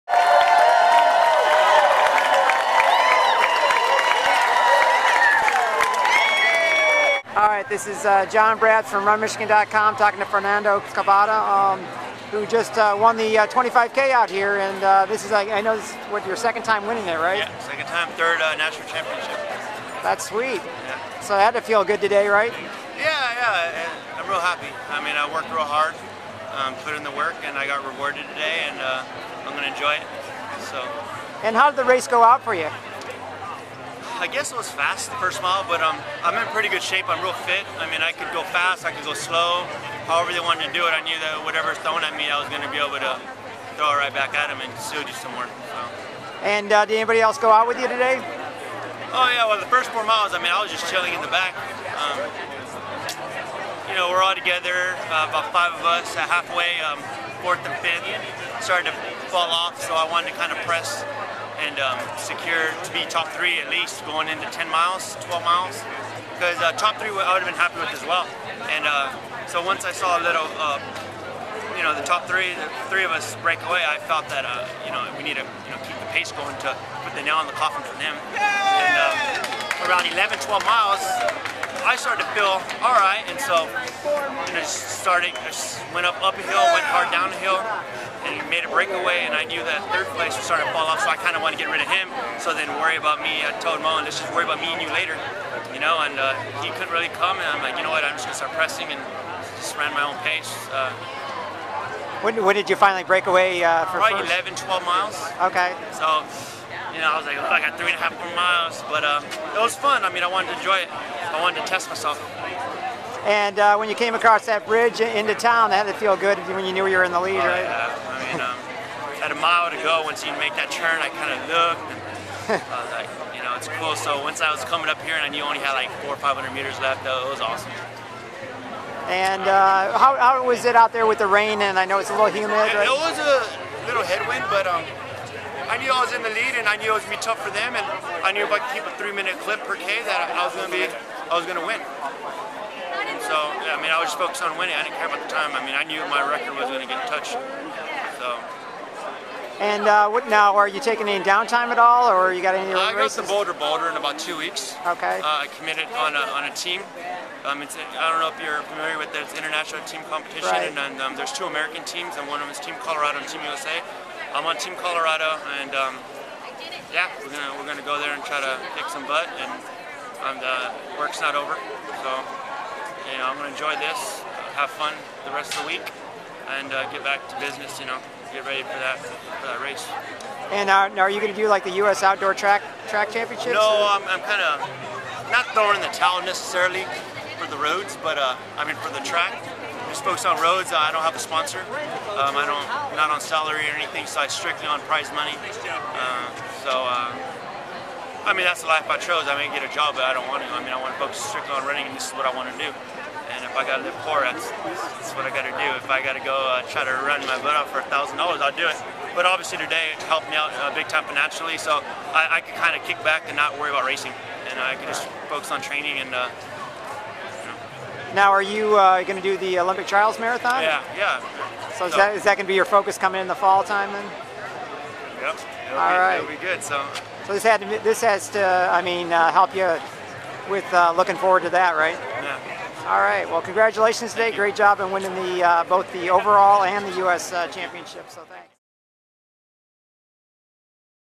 Interview with 2011 Fifth Third River Bank Run Champion Fernando Cabada, by RunMichigan.com
Interview with 2011 Fifth Third River Bank Run Champion Fernando Cabada, by John Brabbs - RunMichigan.com, Grand Rapids, Michigan - May 14th, 2011